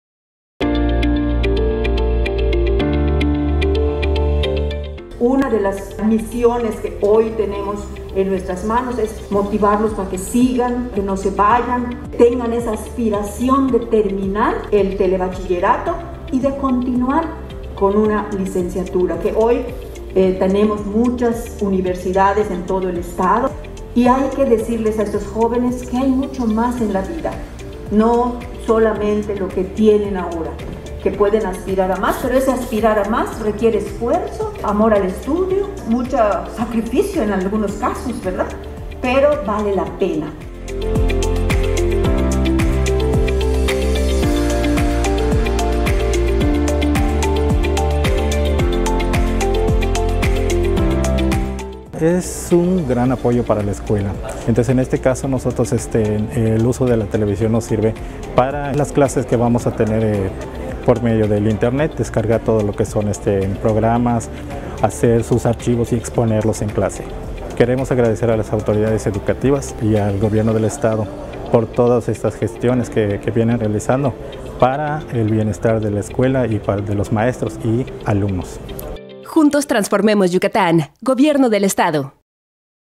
Apoyan trabajo de Telebachilleratos
Personal docente de 27 Telebachilleratos Comunitarios (TBCEY) ya tiene computadoras portátiles nuevas, equipos de oficina multifuncionales y pantallas, con lo que se cumple la indicación del Gobernador Mauricio Vila Dosal de proporcionar herramientas para mejorar las estrategias de enseñanza a distancia e incrementar el alcance de las sesiones didácticas.